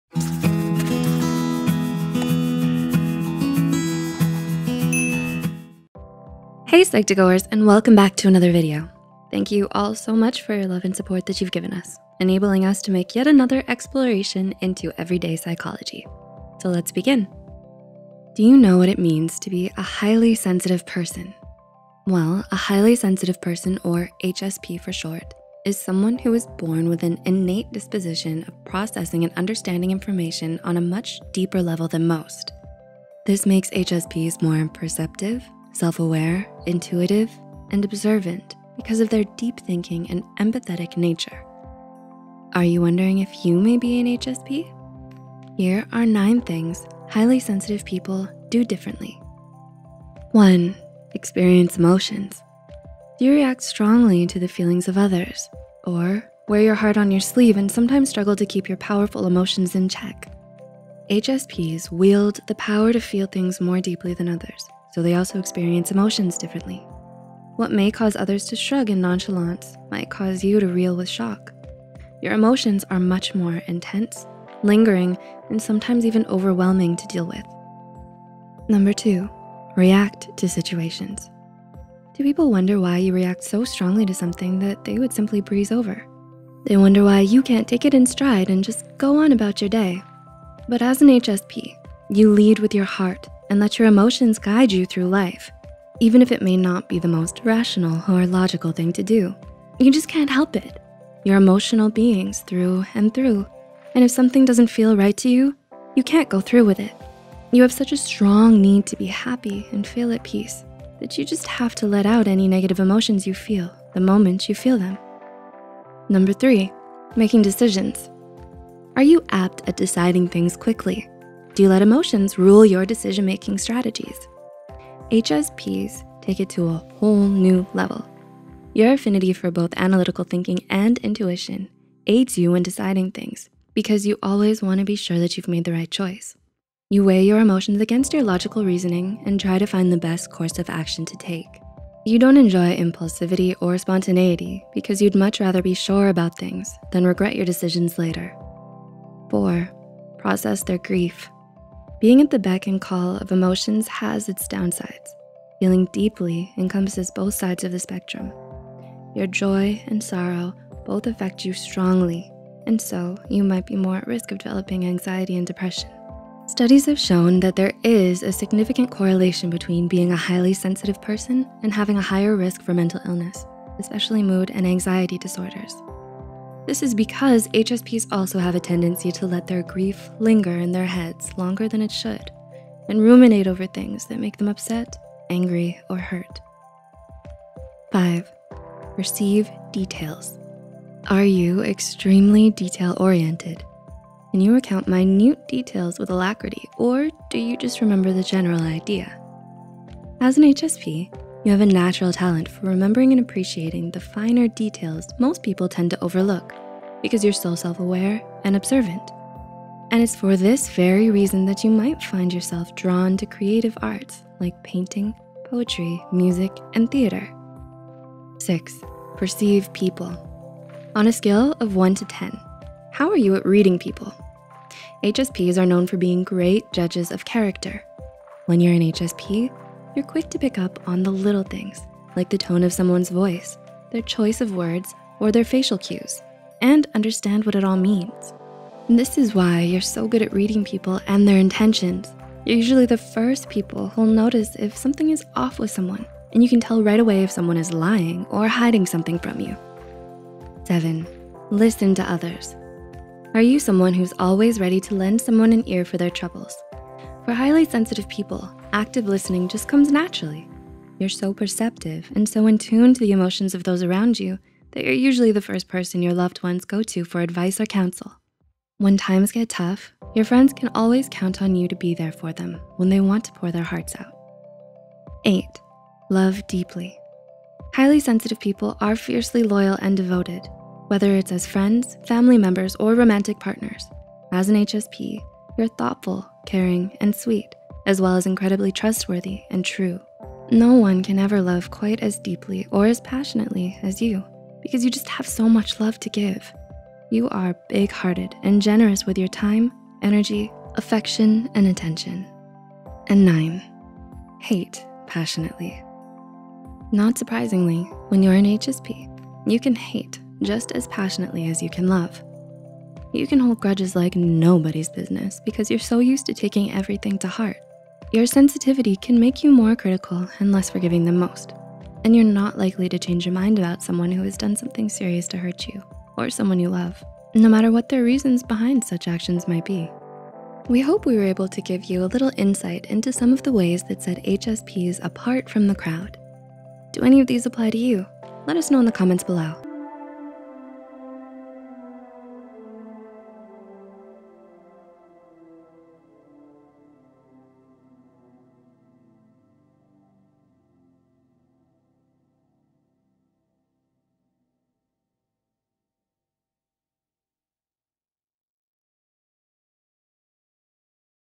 9 Things Highly Sensitive People Do Differently
Highly Sensitive Person (or HSP for short) is someone who is born with an innate disposition of processing and understanding information on a much deeper level than most.  This makes HSPs more perceptive, self-aware, intuitive, and observant because of their deep-thinking and empathetic nature. Recent surveys show that up to 15-20% of the global population are Highly Sensitive People  – that’s as many as 1 in every 5 people all over the world! Still, even with so many of us experiencing the same thing, Highly Sensitive People remain misunderstood by those around them. So many HSPs don’t even realize that they’re HSPs because there is so little awareness about the matter. So, we made a list of things HSPs do differently from others to help you understand more about how HSPs think.

If you think you're an HSP but still not entirely sure yet, we have a video on signs you're a highly sensitive person

Writer: Chloe Avenasa
Script Editor: Rida Batool
Script Manager: Kelly Soong
VO: Amanda Silvera
Animator: Karen Fong
YouTube Manager: Cindy Cheong

Aron, E. N. (1996). Counseling the highly sensitive person. Counseling and Human Development, 28, 1-7.

Aron, E. (2013). The highly sensitive person. Kensington Publishing Corp.

Cooper, T. (2015). The Ordinary Magic of Resilience and the Highly Sensitive Person. The Guilford Press, New York: NY.

Zeff, T. (2004). The Highly Sensitive Person’s Survival Guide: Essential Skills for Living Well in an Overstimulating World. New Harbinger Publications.